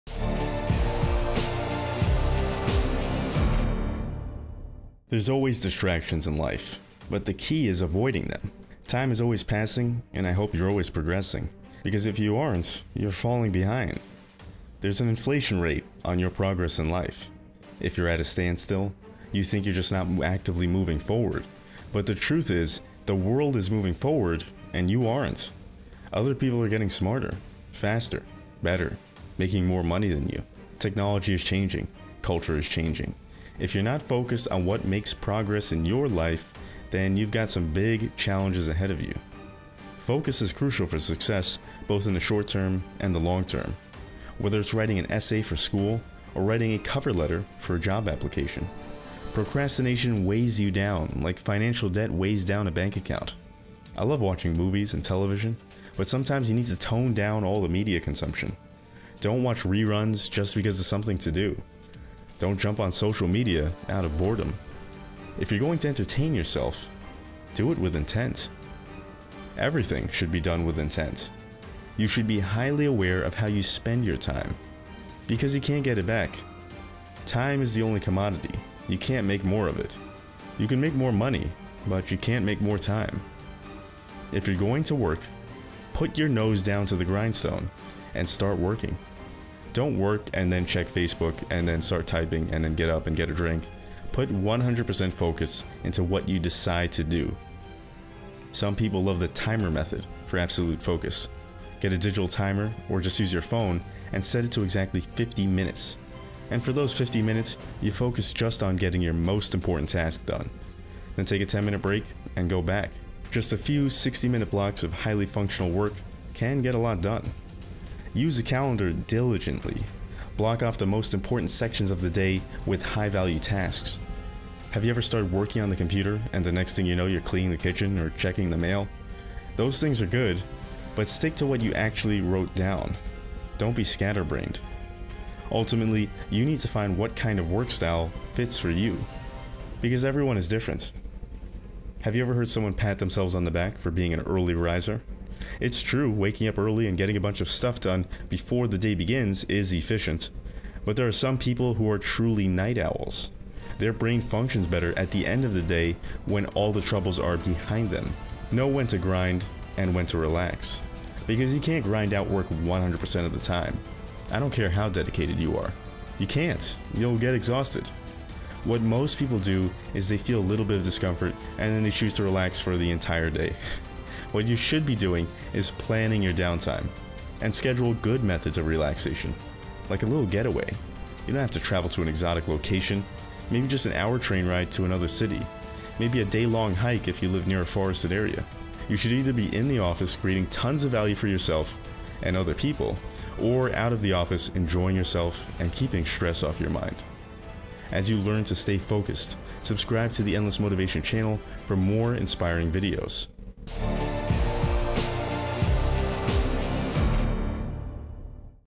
How To Stay Focused - Motivational Video
There’s always distractions in life, but the key is avoiding them.  Time is always passing and I hope you’re always progressing.  Because if you aren’t you’re falling behind.  There’s inflation rate on your progress in life.  If you’re at a standstill, you think you’re just not actively moving forward.  But the truth is the world is moving forward and you aren’t.  Other people are getting smarter, faster, better, making more money.  Technology is changing, culture is changing, if you’re not focused on what making progress for your life than you’ve got some big challenges ahead of you.

Focus is crucial for success both in the short term and the long term.  Whether it’s writing an essay for school or writing a cover letter for a job application.  Procrastination weighs you down like financial debt weighs down a bank account.

I love watching movies and television, but sometimes you need to tone down all the media consumption.  Don’t watch reruns just because it’s something to do.  Don’t jump on social media out of boredom.  If you’re going to entertain yourself, do it with intent.

Everything should be done with intent.  You should be highly aware of how you spend your time.  Because you can’t get it back.  Time is the only commodity you can’t make more.  You can make more money, but you can’t make more time.

If you’re going to work, put your nose down to the grindstone and certain working.  Don’t work and then check facebook, and then start typing and get up and go get a drink.  Put 100% focus into what you decide to do.  Some people love the timer method for absolute focus.  Get a digital timer or just use your phone and set it to exactly 50 minutes.  And for those 50 minutes you focus just on getting your most important task done.  Then take a 10 minute break and go back.  Just a few 60 minute blocks of highly function work like this can get a lot done.

Use a calendar diligently.  Block off the most important sections of the day with high value tasks.  Have you ever started working on the computer and then next thing you know you’re cleaning the kitchen or checking the mail.  Those things are good, but stick to what you actually wrote done.  Don’t be scatter brained.

Ultimately you’ll need to find what kind of work style fits for you.  Because everyone is different.  Have you ever heard someone pat themselves on the back for being an early riser.  It’s true, waking up early and getting a bunch of stuff done before the day begins is efficient, but there are some people who are truly night owls.  Their brain functions better at the end of the day, when all the troubles are behind them.

Know when to grind and when to relax.  Because you can’t grind out work 100% of the time.  I don’t care how dedicated you are.  You can’t.  You’ll get exhausted.  What most people do is they feel a little bit of discomfort and they choose to relax for the rest of the day.  What you should be doing is planning your downtime.  And schedule good methods of relaxation.  Like a little getaway.  You don’t have to travel to an exotic location, maybe just an hour train ride to another city.  Maybe a day long hike if you live near a forested area.
You should either be in the office creating tons of value for yourself and other people or out of the office enjoying yourself and keeping stress off your mind.